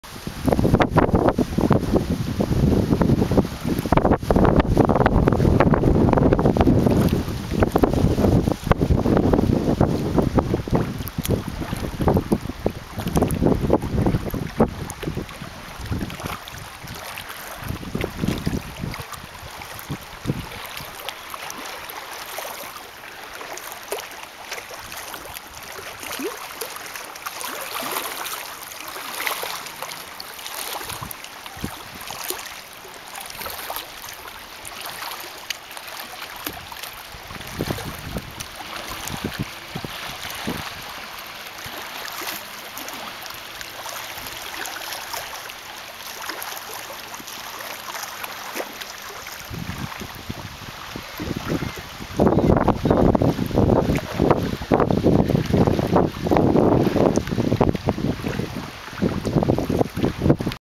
Lulu View Cottage Summer 2009
This is the cottage I went to this summer for a week. This is the view from the bay just off our dock. Beautiful Place